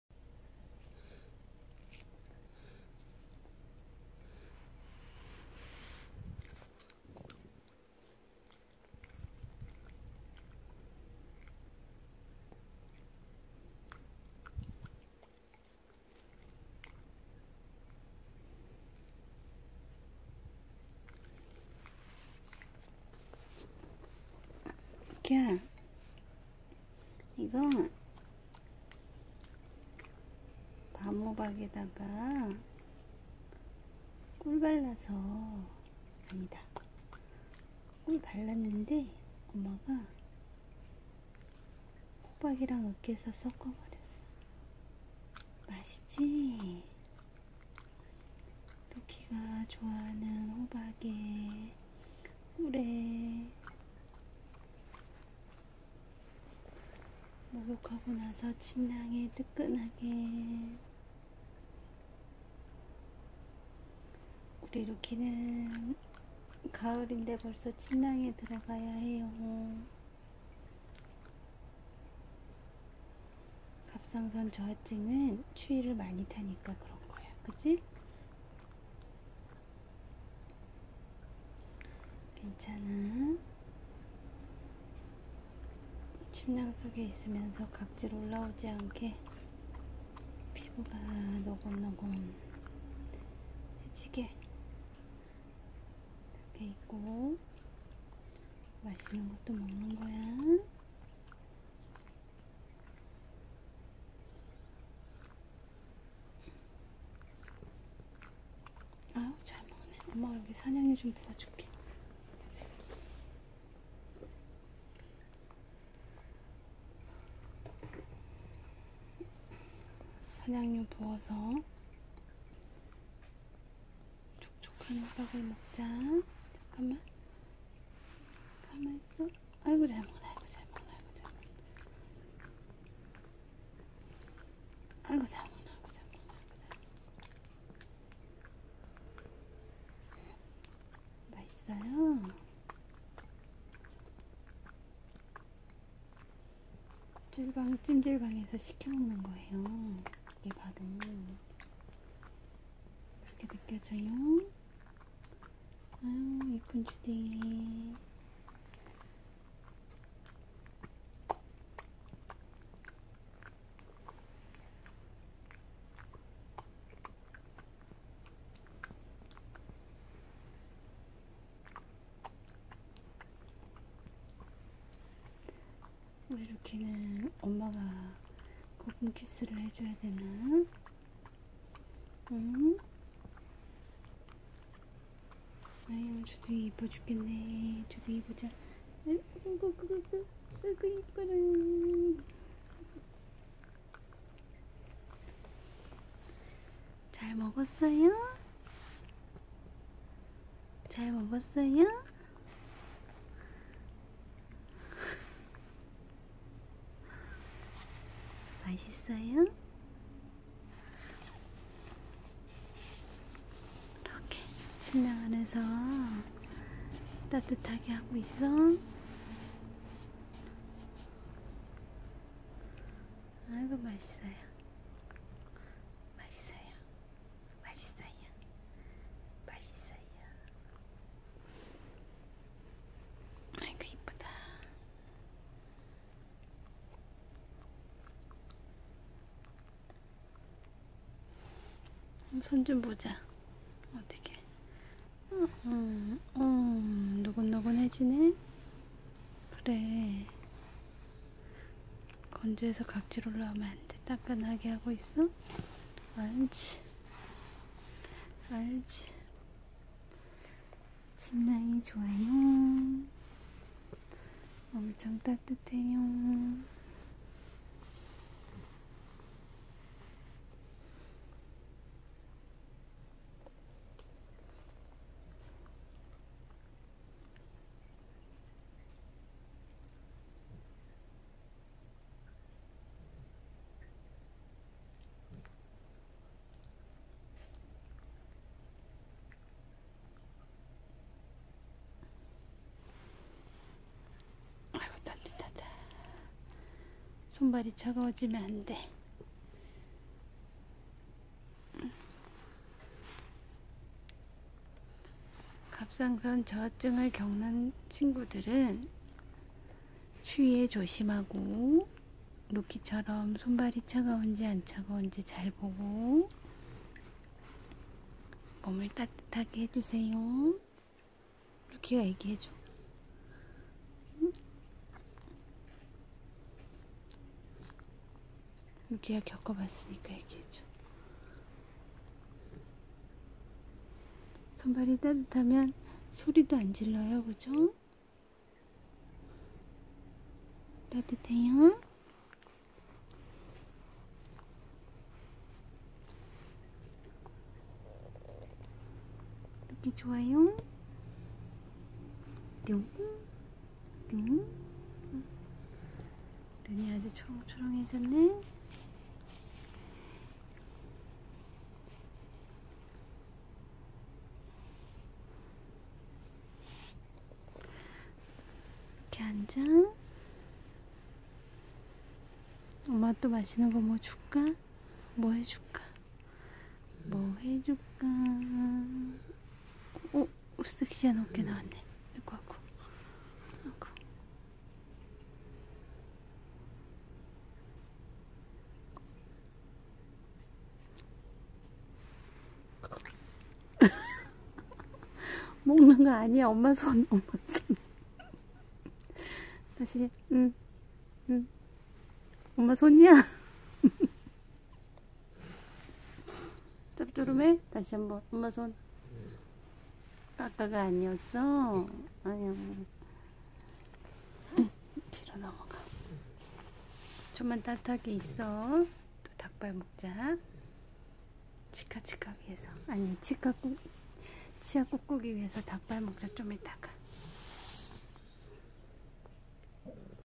노견 목욕후 따뜻하게 해요^^체온, 면역력 올리기
목욕후 뜨끈한 침낭과 꿀호박 먹타임